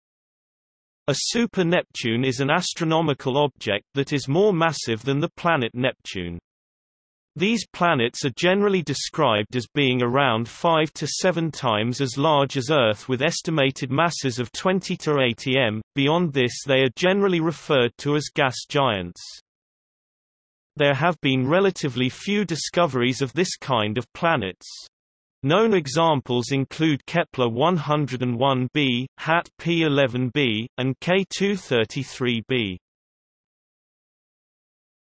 Super-Neptune | Wikipedia audio article
- Socrates

SUMMARY
A super-Neptune is an astronomical object that is more massive than the planet Neptune. These planets are generally described as being around 5–7 times as large as Earth with estimated masses of 20–80 M⊕, beyond this they are generally referred to as gas giants.
There have been relatively few discoveries of this kind of planets. Known examples include Kepler-101b, HAT-P-11b, and K2-33b.